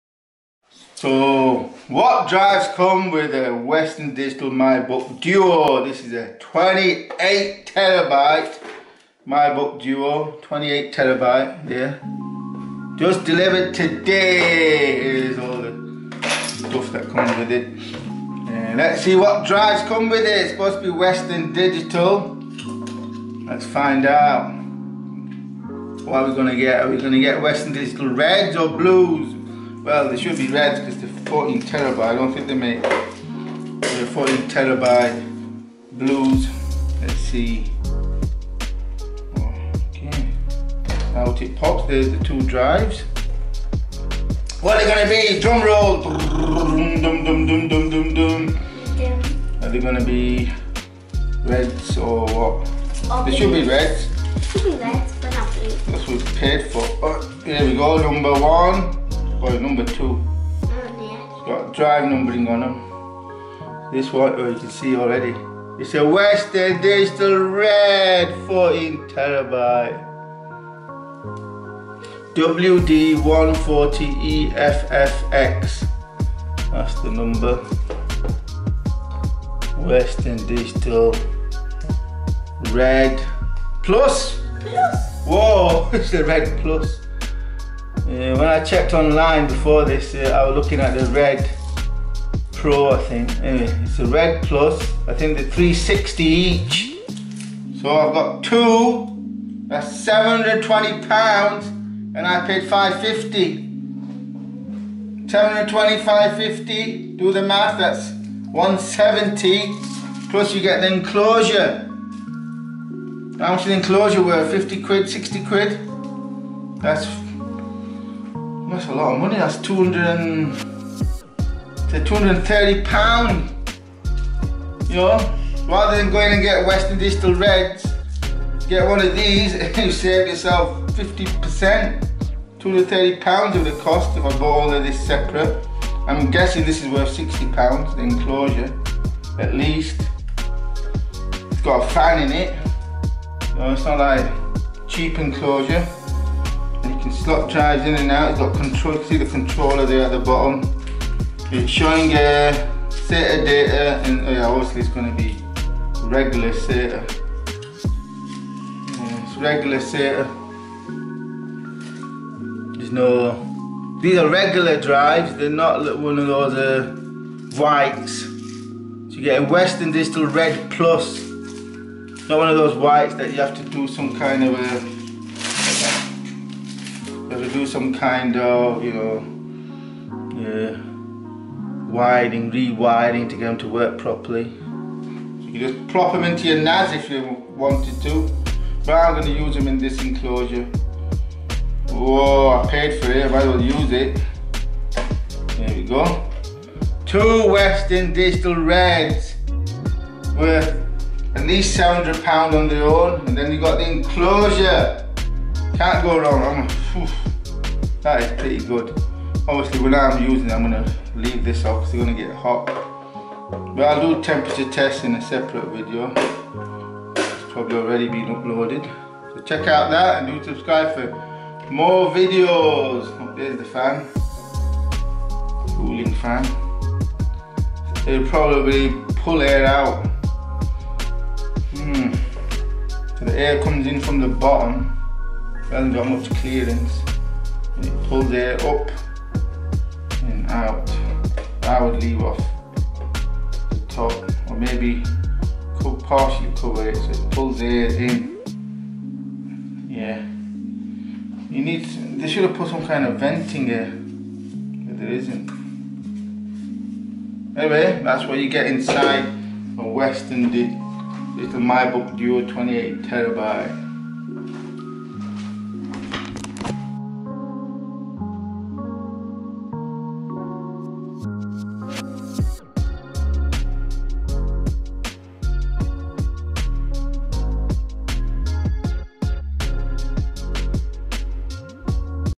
WD MY Book DUO Drives Lottery - What Disks are Inside - Red or Blue - 28tb Model
What Drives are inside a Mybook DUO is it a red or blue find out as I open up a 28TB My Book DUO enclosure and find out.

By HGR

MY Book DUO - WHat Drives Inside - Red or Blue - 28tb Model